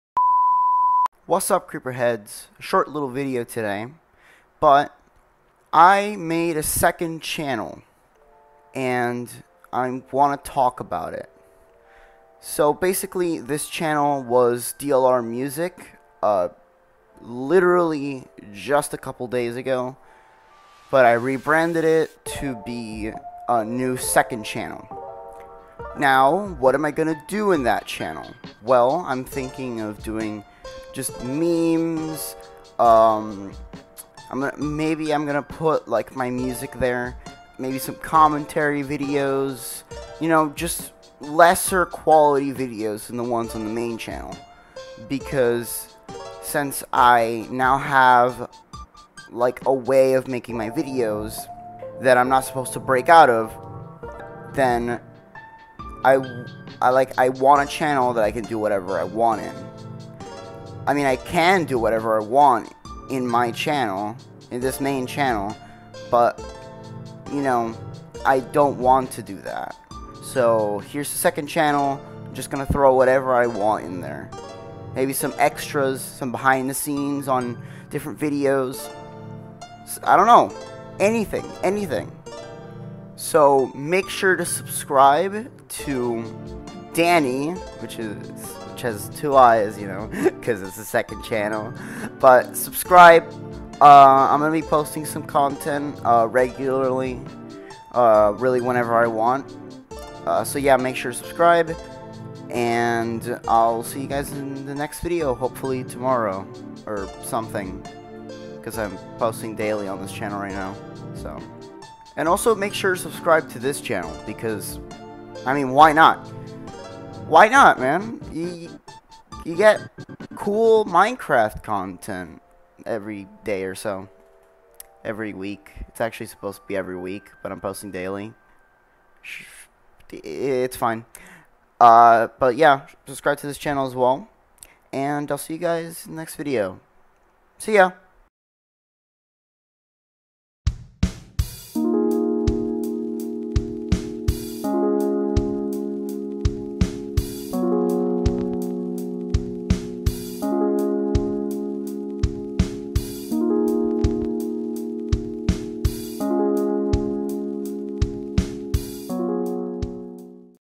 I made a Second Channel.
I made a Second Channel. Yes, I finally found a use for my defunct second channel, so I'm going to upload some stuff there and it's going to be great.